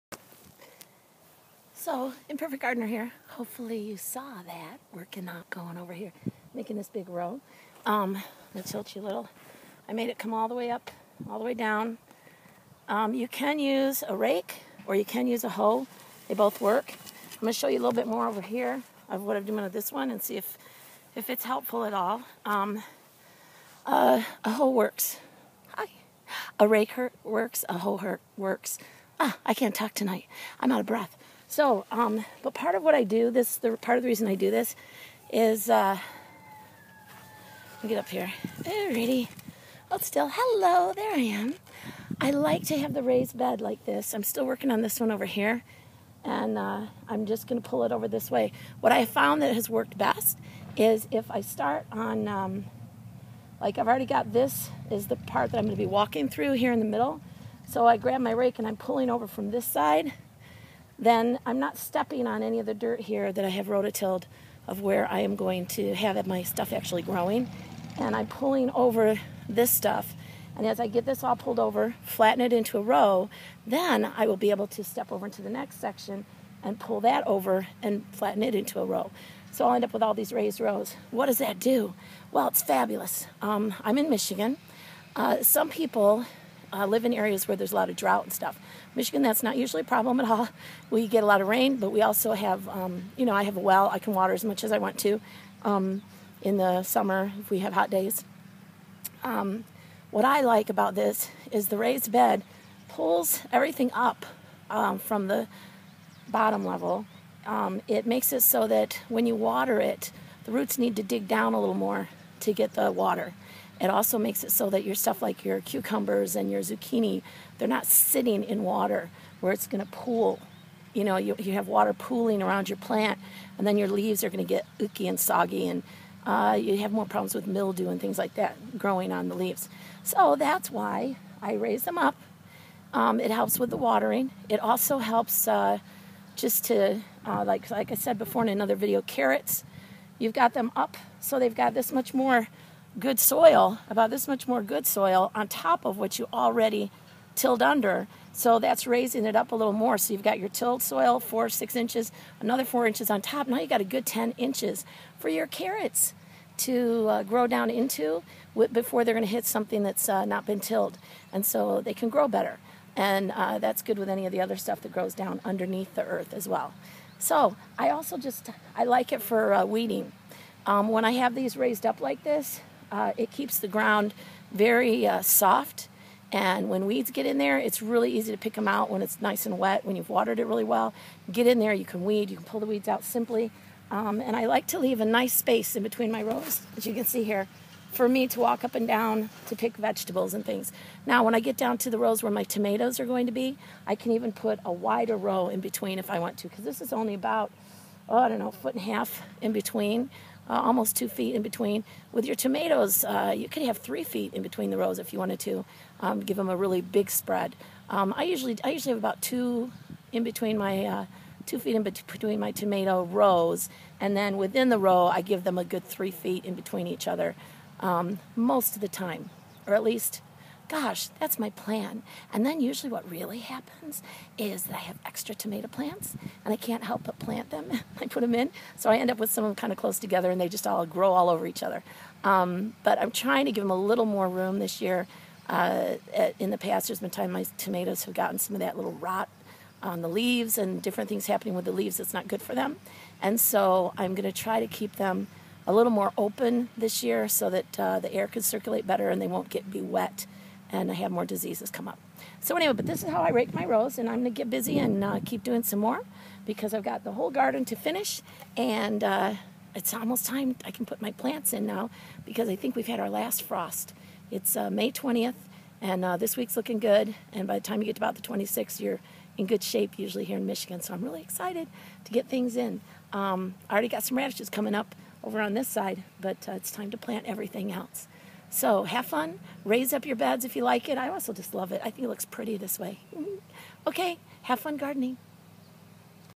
How and Why I use Raised Rows/Beds in the Garden
How and Why I use raised rows/beds in the Garden. Growing your own vegetables is fun and anyone can do it. You don't have to be perfect.